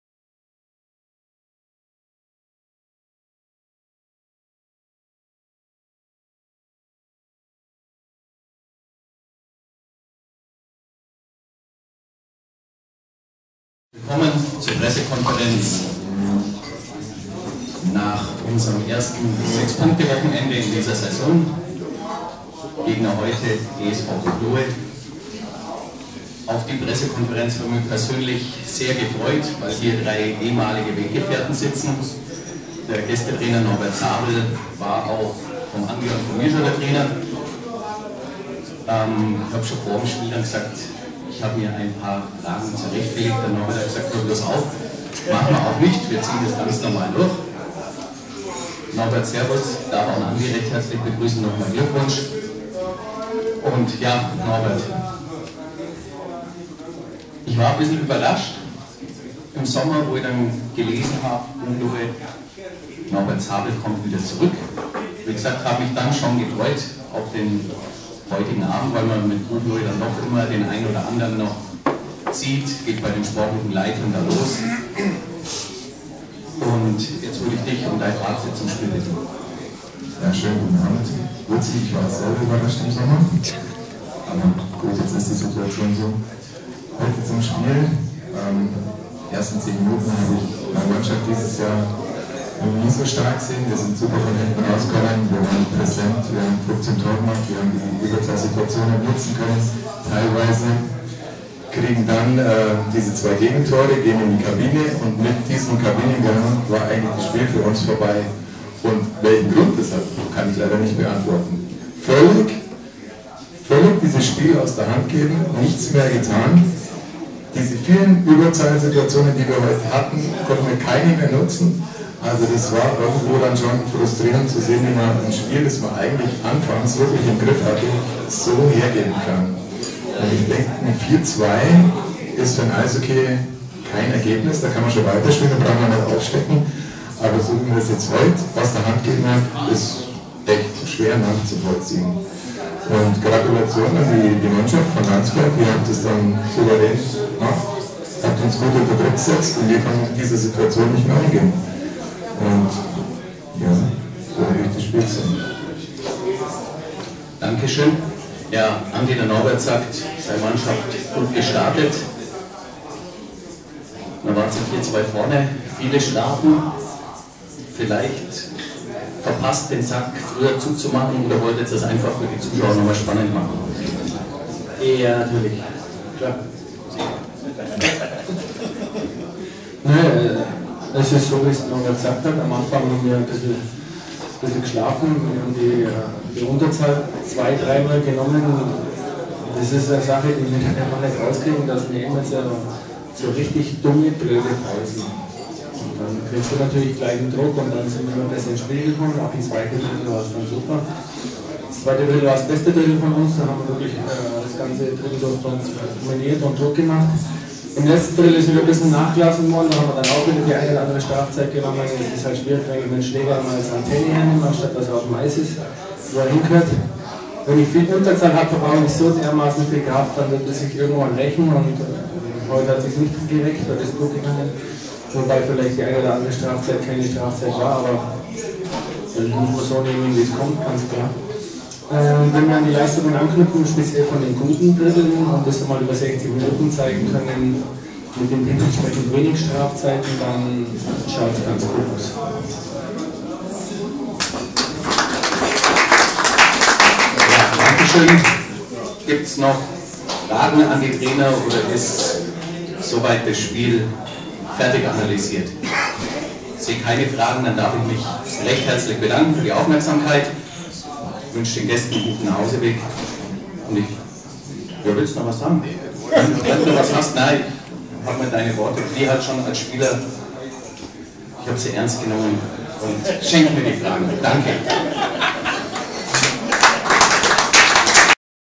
PK: 8. Spieltag (01.11.2015): HC Landsberg - ESV Buchloe
Im Lokalderby gegen den HC Landsberg mussten die Buchloer Piraten am Ende ohne Punkte die kurze Heimreise antreten. Ein starkes Anfangsdrittel reichte bei der 5:3 Niederlage nicht, um zählbares zu entführen.
Hier die Pressekonferenz nach dem Derby.